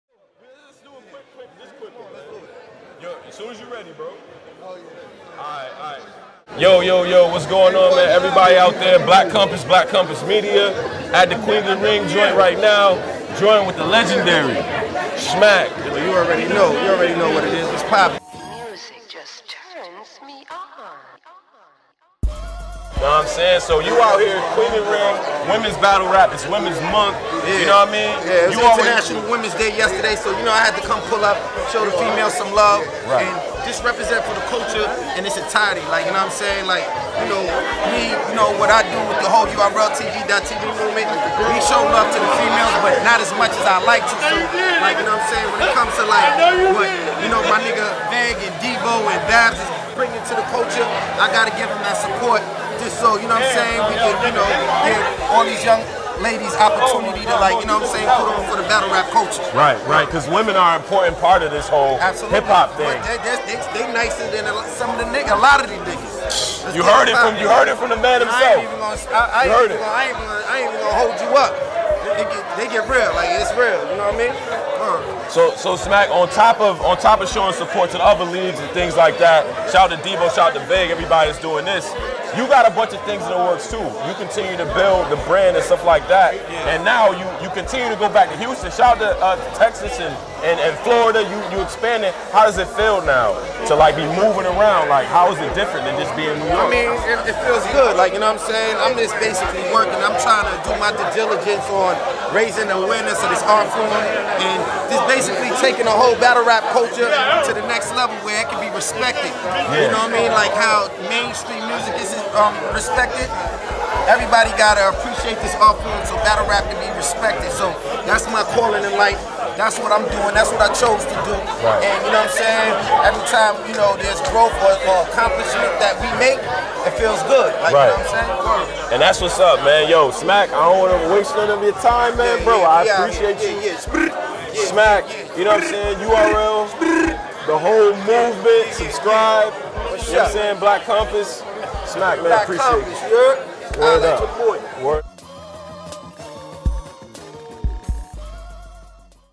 SMACK Says "Some Of These Ladies Will Kill The Guys", Addressing Queen Of The Ring At WTT3
BATTLE RAP LEGEND SMACK talks about the importance of women in battle rap, URL relatioship with Queen of the Ringg and details URL moves for 2019!

ORDER THE PPV WATCH THE THRONE 3 BY QUEEN OF THE RING